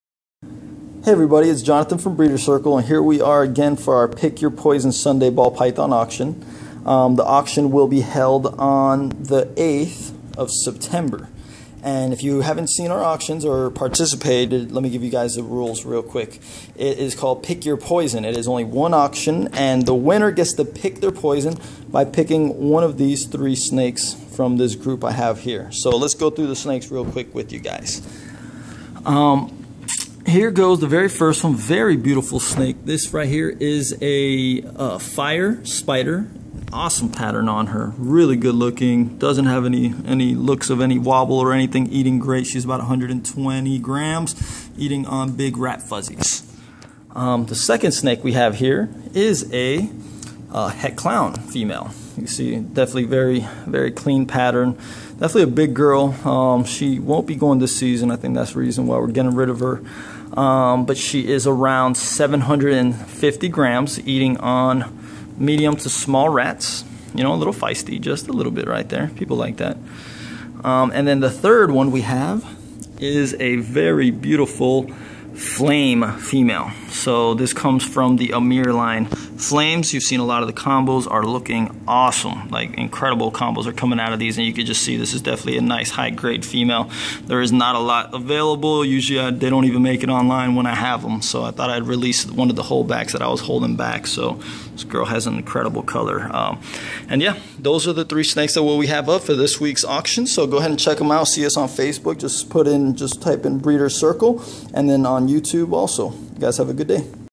Pick Your Poison Sunday Ball Python FACEBOOK Auction- September 8th 2013.
Our Pick Your Poison Sunday Ball Python FACEBOOK Auction- September 1st 2013.
Up for Auction!
-2013 Fire Spider female at 120g eating on big rat fuzzies
-2012 100% het Clown female at 750g eating on small/medium rats.
-2013 Flame (het Black Eyed Leucistic)  female at 105g eating on rat fuzzies

Auction starts at 12PM and ends at 10PM eastern standard time

Shipping in United States is a flat fee of 40.00 for auctions.
International shipping is available. And delivery to Snke Day in Holland in October  and then Hamm, Germany in December for 150.00